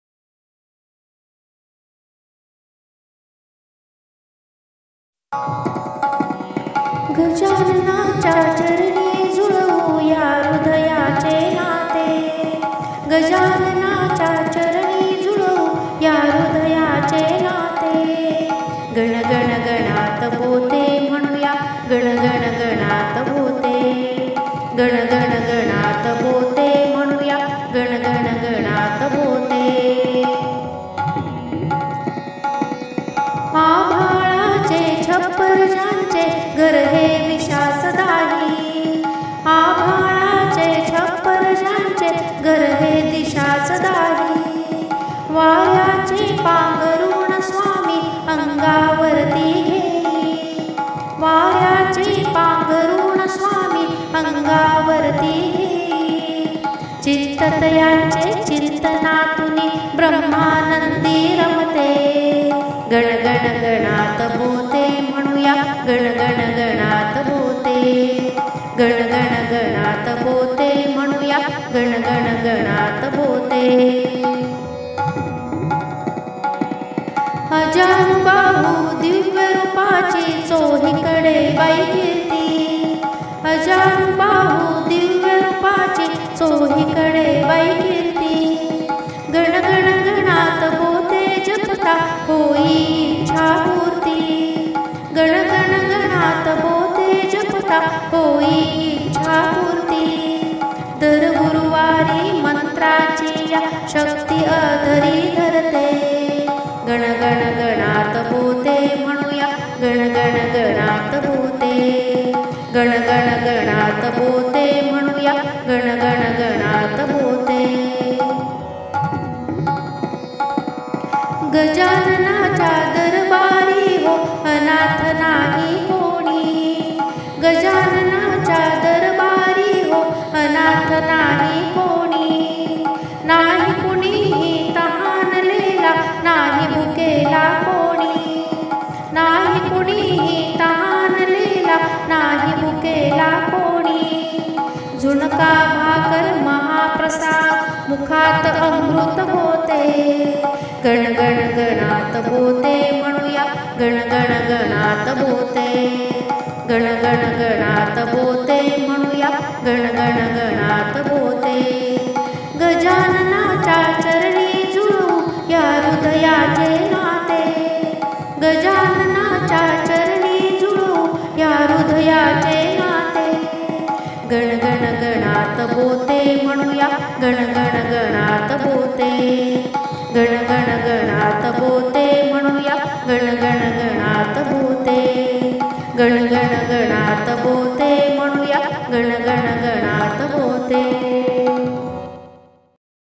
ll प्रगट दिन स्पेशल गजानन महाराज नवीन bhajan song by मंजुश्री दवणे
@manjushreedavane 3 मार्च 2024... गजानन महाराज प्रगट दीना निमित्त अतिशय सुमधुर आणि नवीन गाणे... स्वर... मंजुश्री दवणे